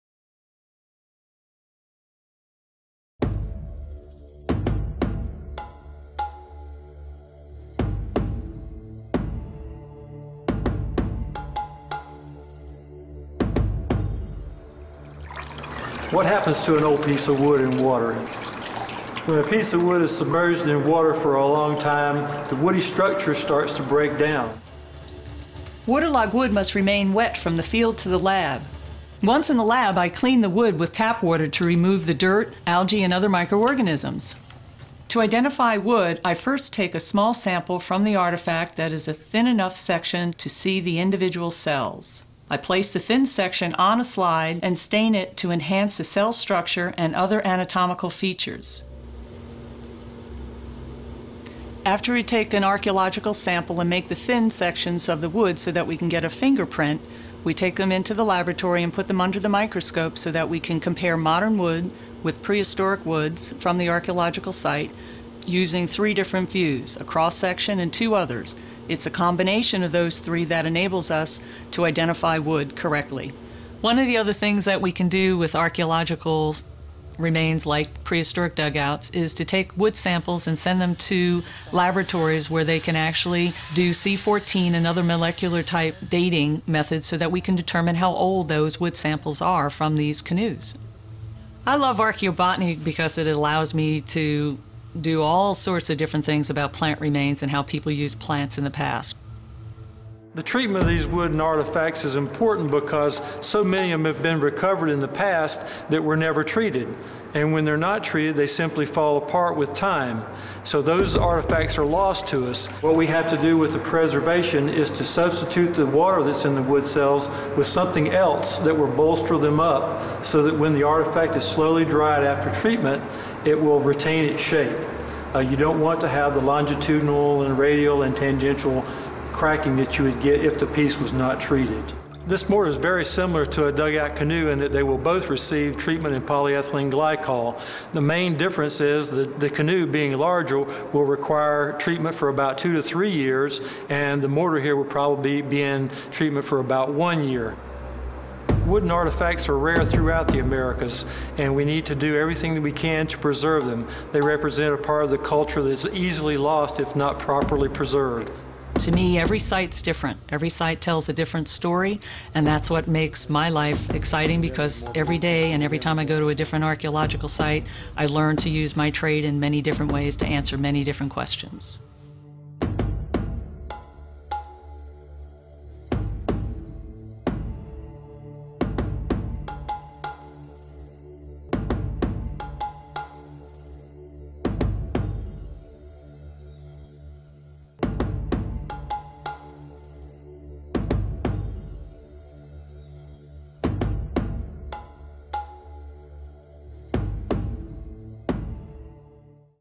Dugout Canoes-Canoes in the Lab
This video shows how scientists carefully study and preserve delicate dugouts from the past.